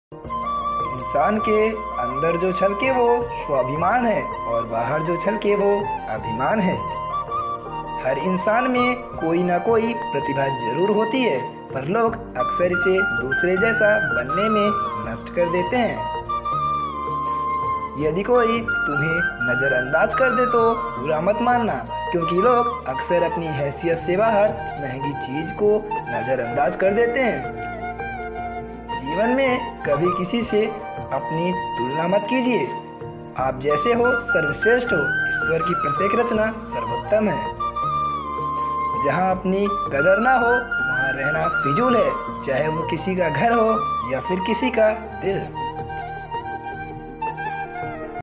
(हर मनुष्य में कोई ना कोई गुण जरूर होता है)There is definitely some quality in every human being.🤗
Hello my YouTube family friends 👋
      How are you 😊
i hope everyone is well
   (हर मनुष्य के अंदर कोई ना कोई गुण जरूर होता है)
  There is definitely some quality in every human being. 🤗
    Today's topic is self respect, about which we are talking, we hope you have liked this video, if you have liked it, then please like the video and give us your suggestion by commenting.

guys please Watch my video and please support me 👍

Ñõtê. Friends, this video is made to motivate you, no copied music of any kind is used on this video.